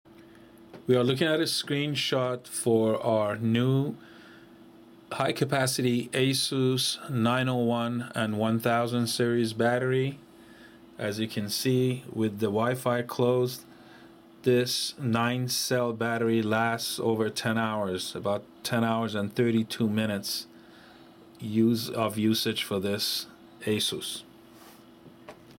Asus EEEPC 901 1000 1000h 7.4V 10400mAh High Capacity Replacement Battery by ShopXtreme
Our High Capacity Replacement battery for Asus EEEPC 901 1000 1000h 7.4V 10400mAh gives you over 10.5 HOURS of run time! Check it out! More info at ShopXtreme.com!